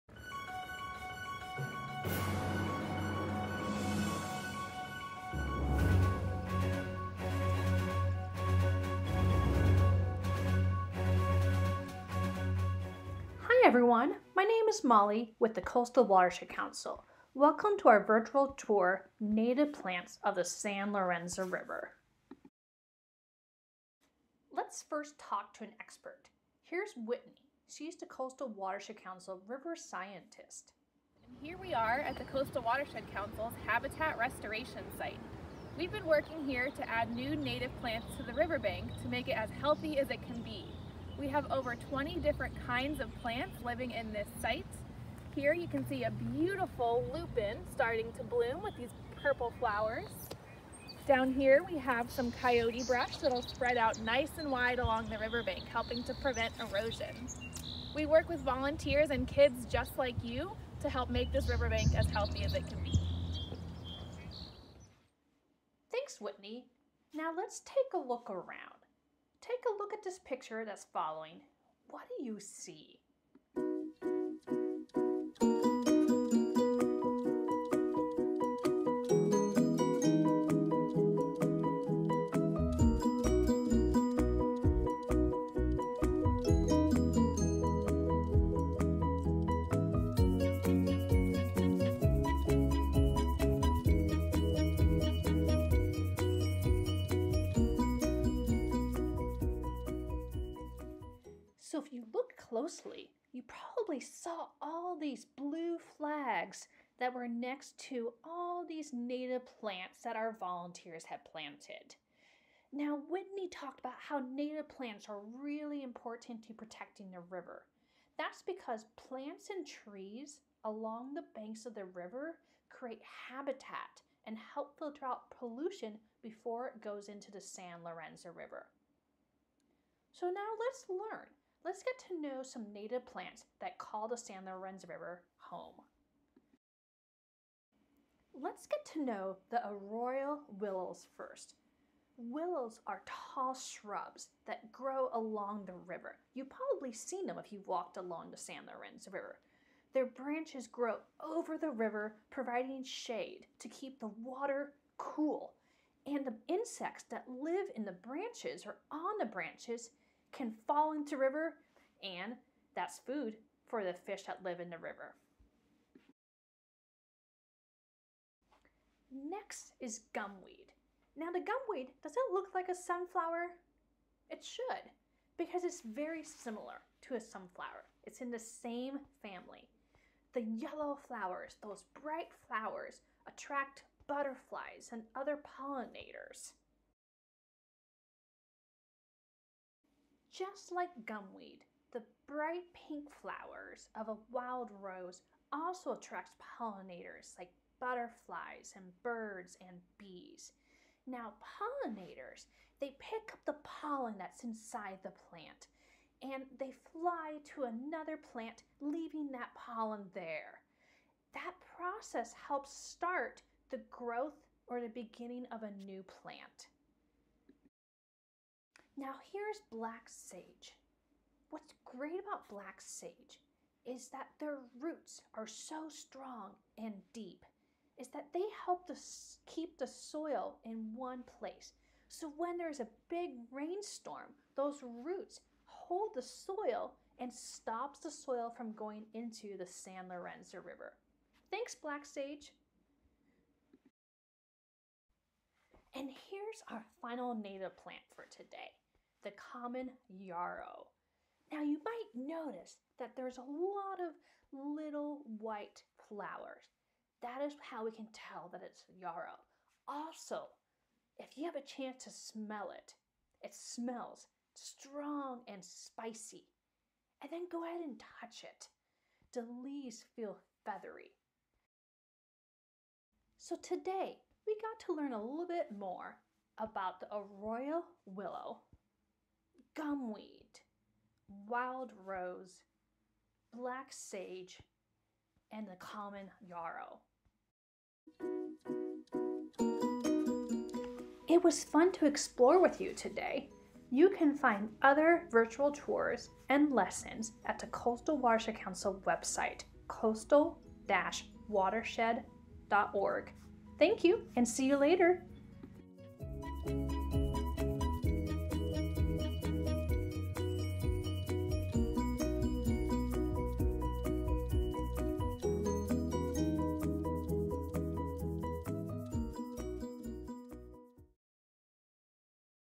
Interactive Virtual Tour: Native Plants of the San Lorenzo River
This interactive virtual tour introduces you to native plants that call the San Lorenzo River home and explores how native plants are important in keeping the San Lorenzo River healthy.